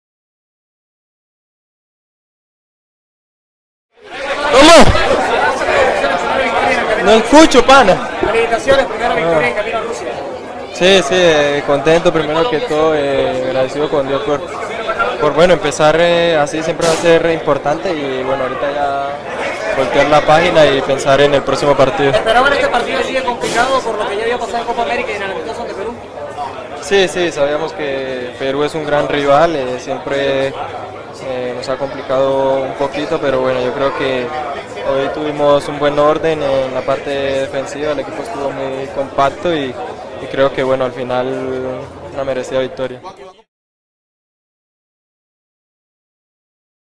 JUAN GUILLERMO CUADRADO
JUAN GUILLERMO CUADRADO Y SUS IMPRESIONES DESPUES DEL PARTIDO COLOMBIA vs PERRU EN BARRANQUILLA